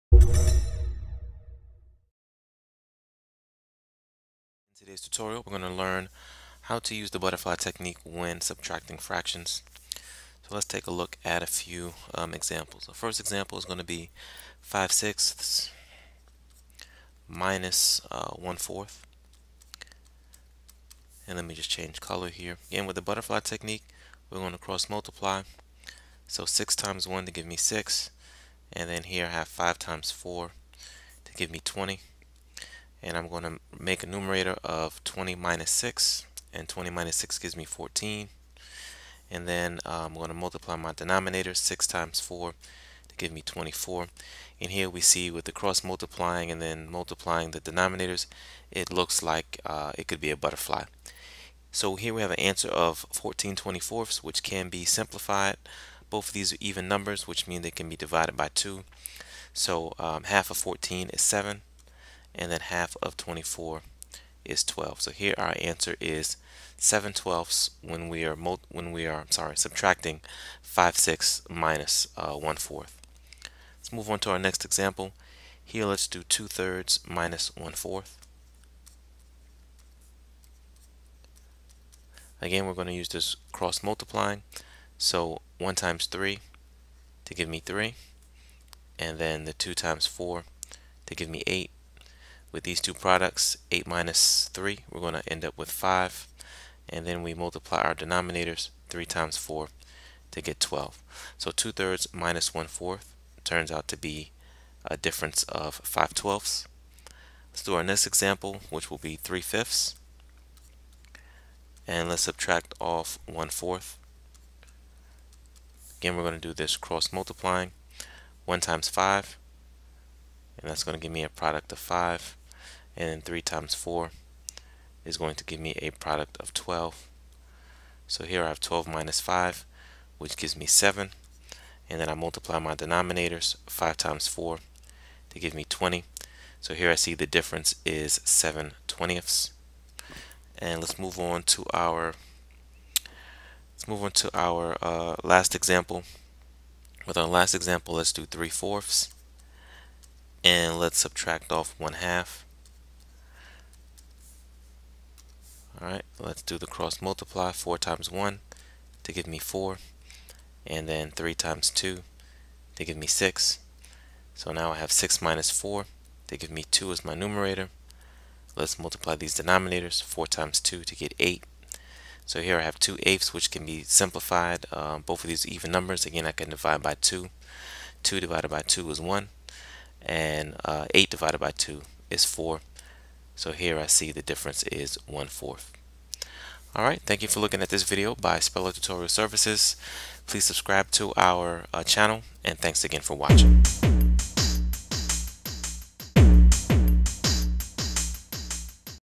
How to subtract fractions using the butterfly method
In this tutorial students will learn how to subtract fractions using the butterfly method.